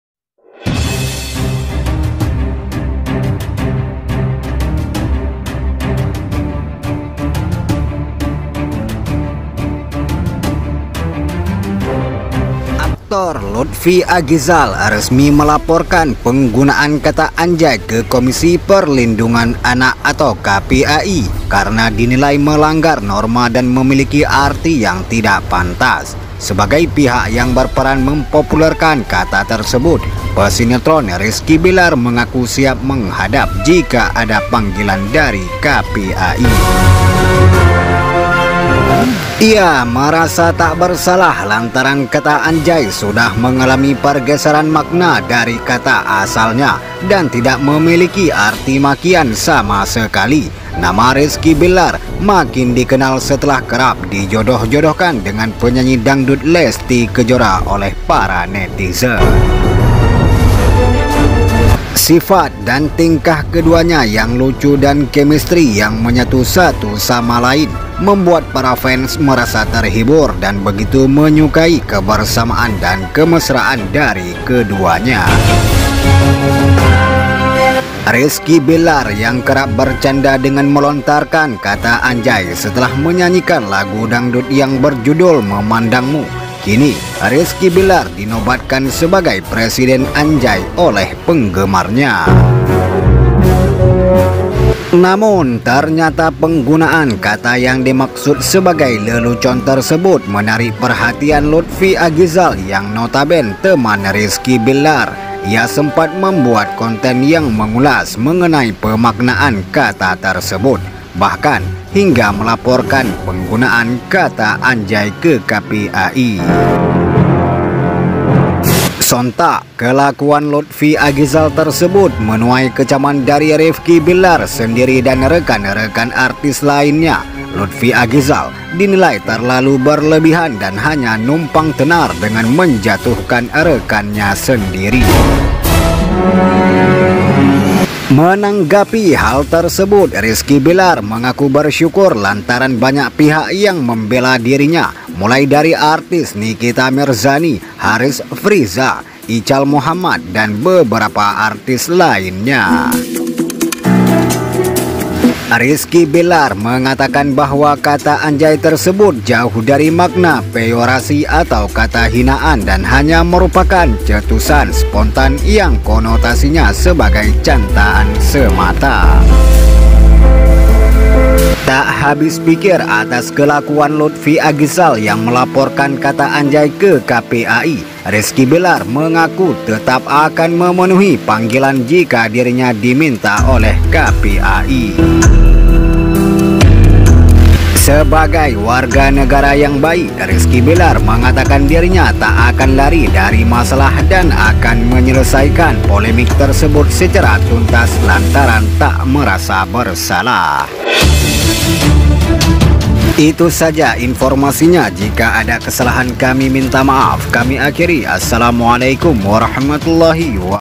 SIKAP RIZKY BILLAR SAAT JUMPA DENGAN LUTFI DI PUJI BANYAK PIHAK, MESKI SUDAH DILAPORKAN KE KPAI
RIZKY BILLAR DI BANGGAIN AYAH LESTY TERKAIT KASUSNYA DENGAN LUTFI, DIA CALON MENANTU YANG BAIK.